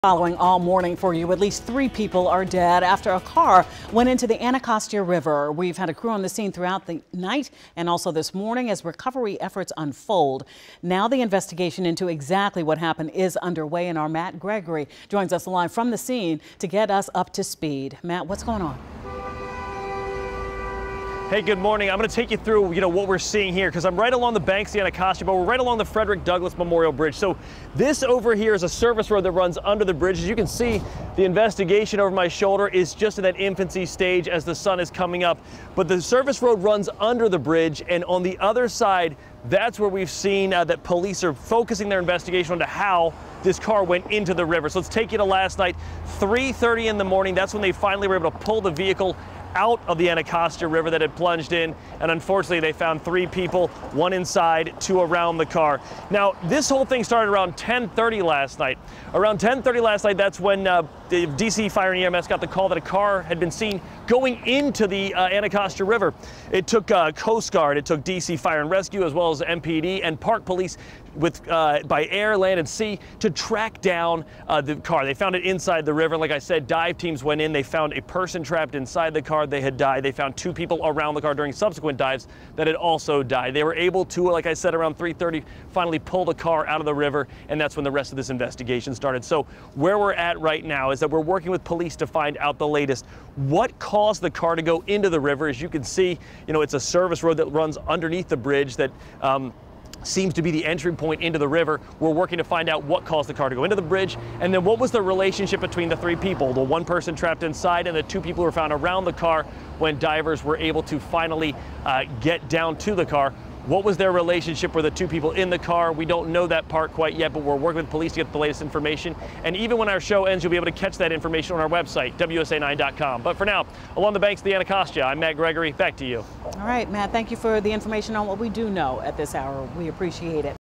Car into Anacostia river leaves 3 dead
Police are still investigating exactly what happened.